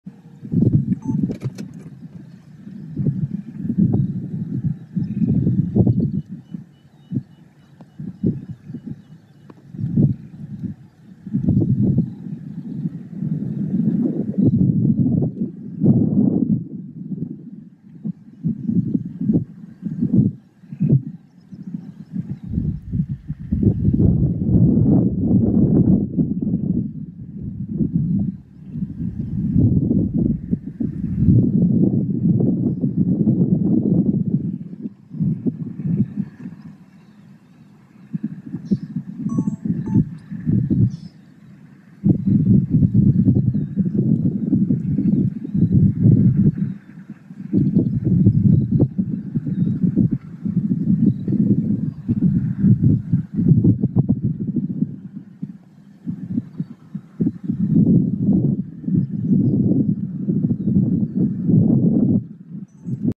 Elands on safari with @explorewildlifeafrica-adve917 in the Masai Mara National Park 2024
On Safari in the Masai Mara National Reserve, encounters of these Elands is always a great sighting and this morning is fantastic with @explorewildlifeafrica-adve917